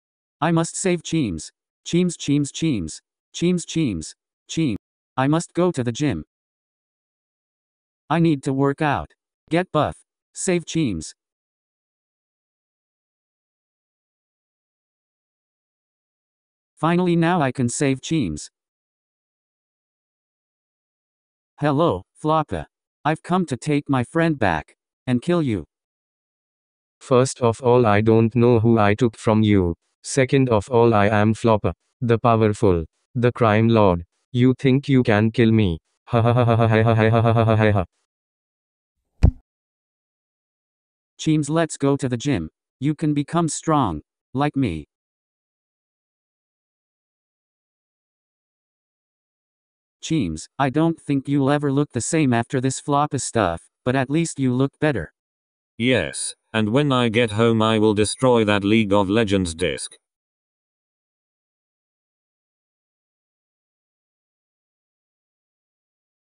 How Doge Became Buff
This video is the part two to the Cheems origin video. It is also the video that explains how Doge became buff.